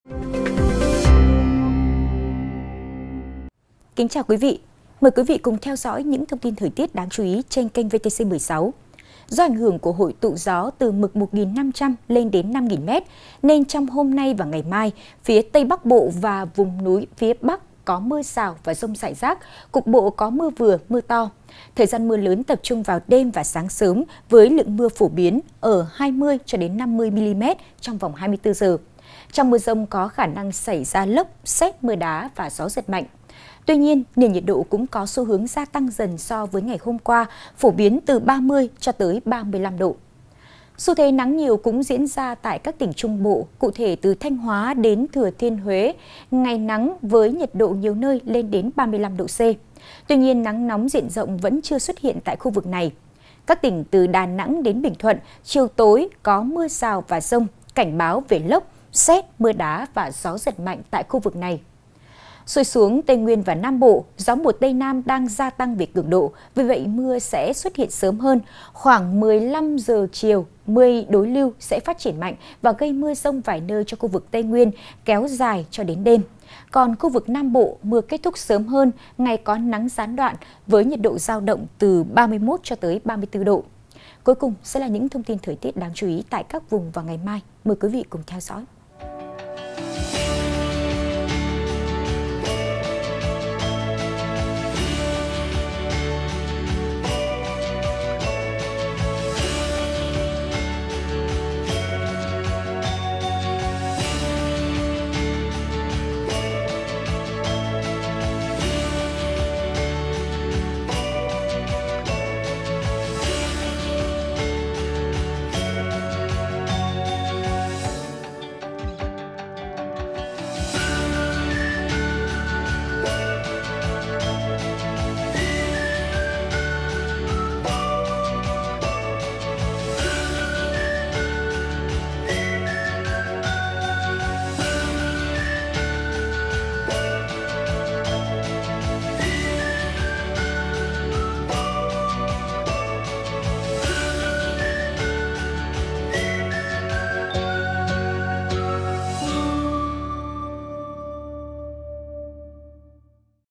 Dự báo thời tiết 31/05/2020 | Bắc Bộ: Cảnh báo giông, lốc về đêm | VTC16
VTC16 | Dự báo thời tiết phía Tây Bắc Bộ và vùng núi phía Bắc có mưa rào và giông rải rác, thời gian mưa lớn tập trung vào đêm và sáng sớm. Trong mưa giông có khả năng xảy ra lốc, sét, mưa đá và gió giật mạnh.